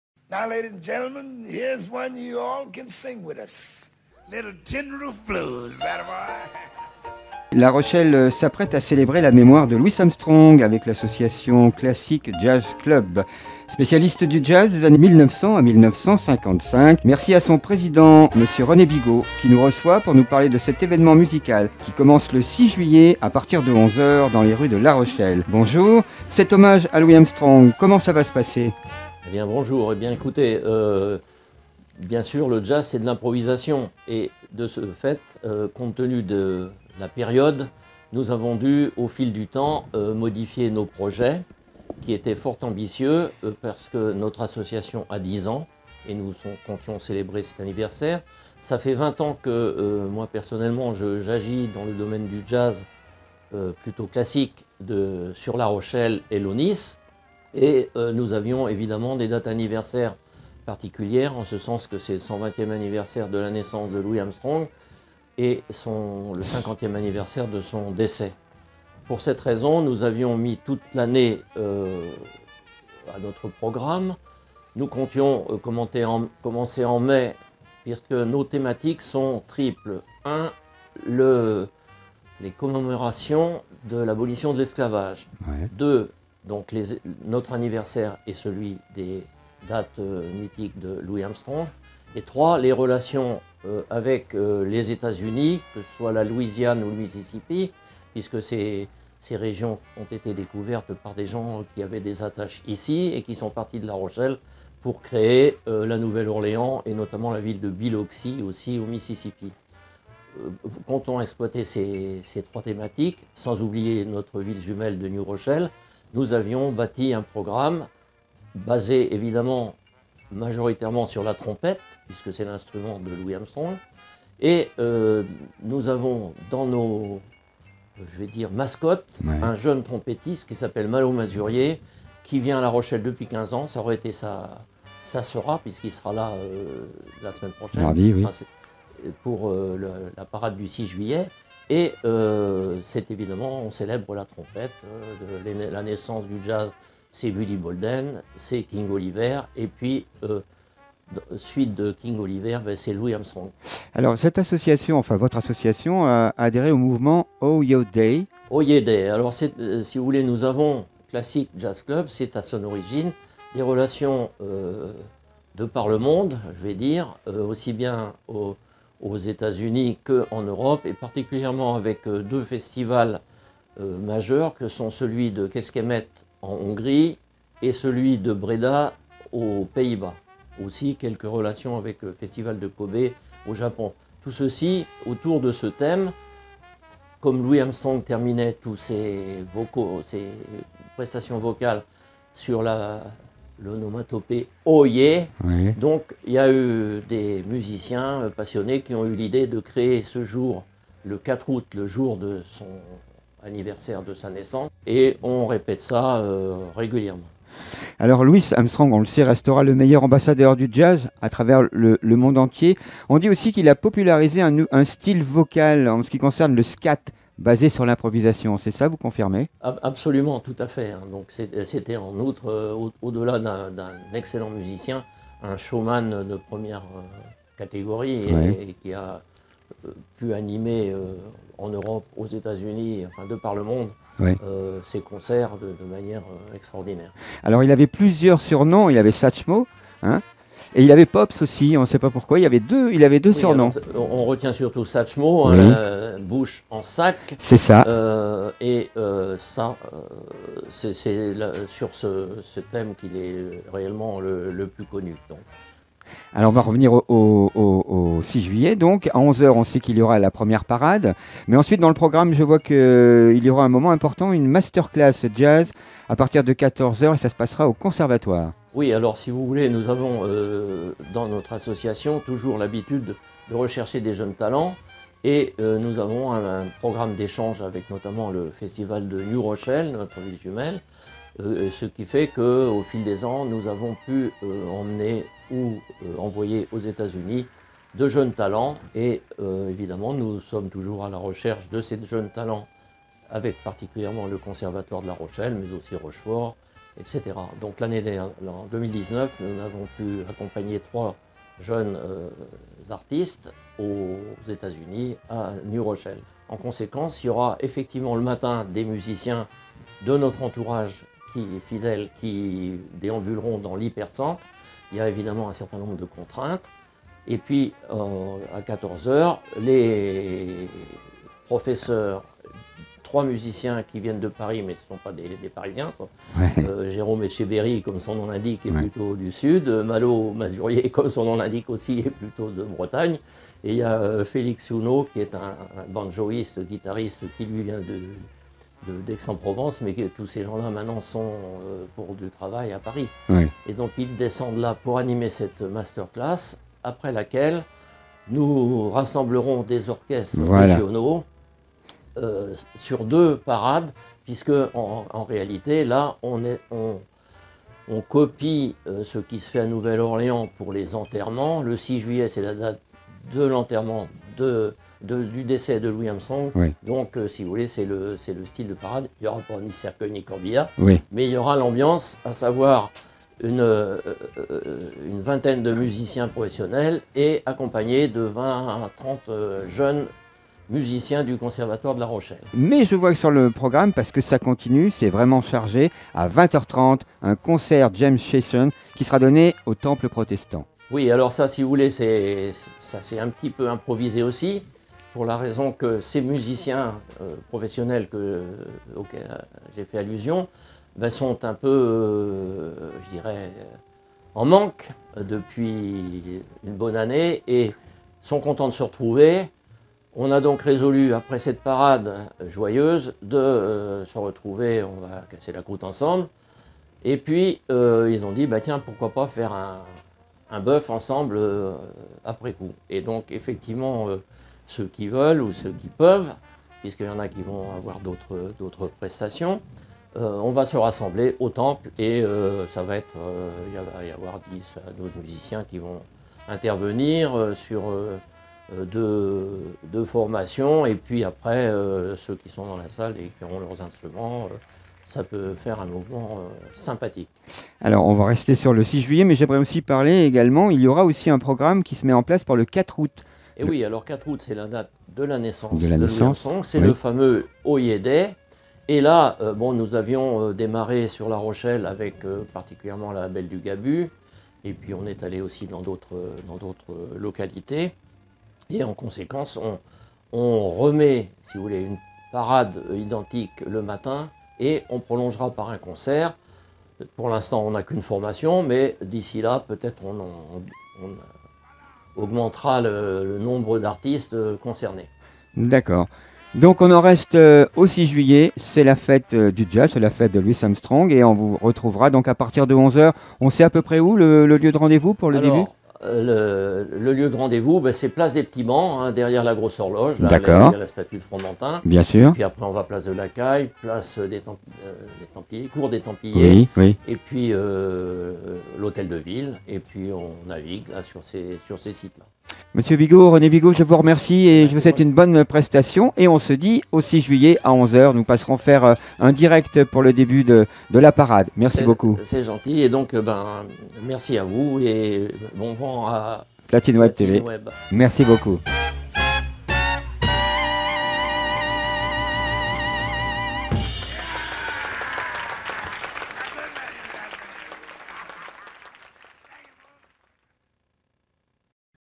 JAZZ A LA ROCHELLE ! Le Classic Jazz club célèbre Louis Armstrong le 6 juillet à LaRochelle..
JAZZ A LA ROCHELLE !
Le Classic Jazz club célèbre la mémoire de Louis Armstrong le 6 juillet dans les rues de LaRochelle. adhérant au mouvement ‘‘Oh Yeah Day’‘, le but est de rendre hommage au grand jazzman..
PlatineWeb a rencontré René Bigot Président de l’association, il nous présente cet événement..